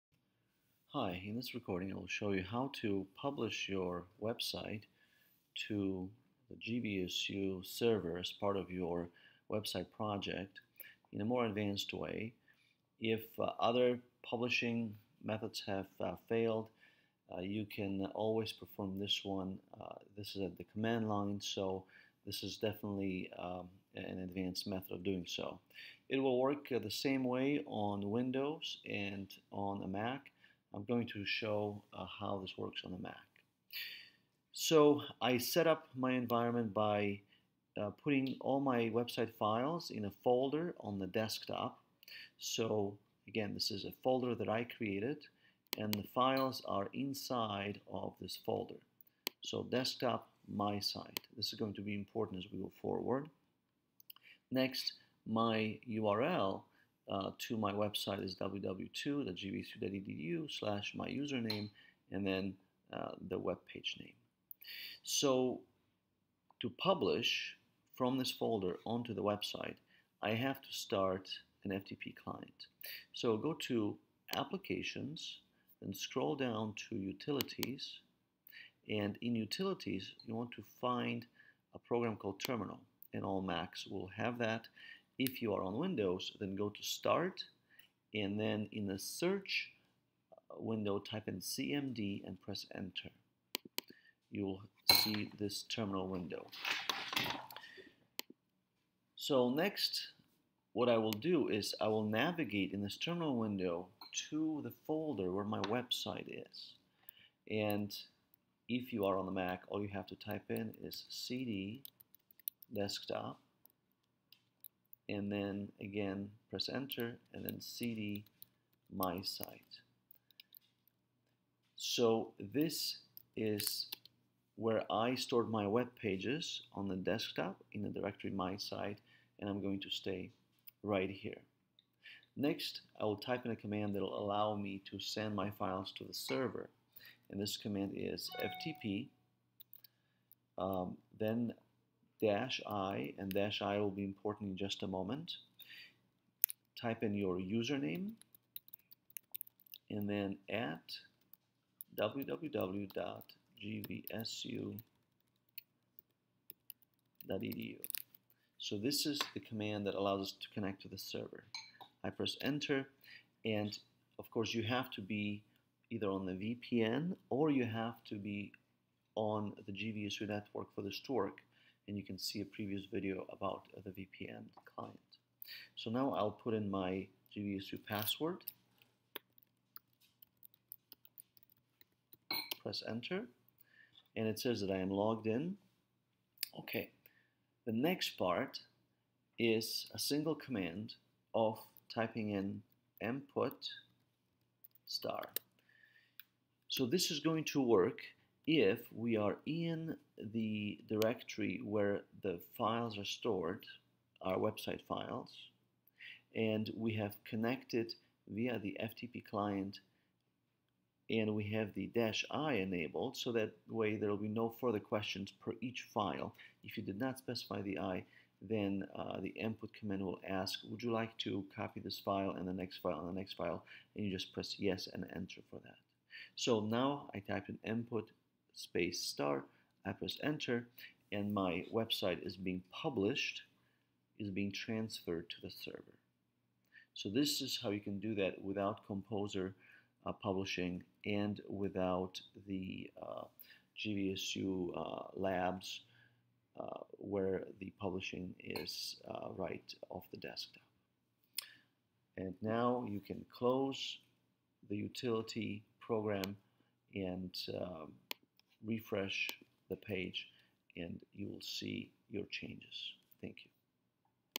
Website project: Advanced method of publishing
This method of publishing your website involves the command prompt.  It will work both on Windows and Mac.  You do not need to install any ftp clients.

Commands used in the video:

cd Desktop
cd mysite
bin
mput *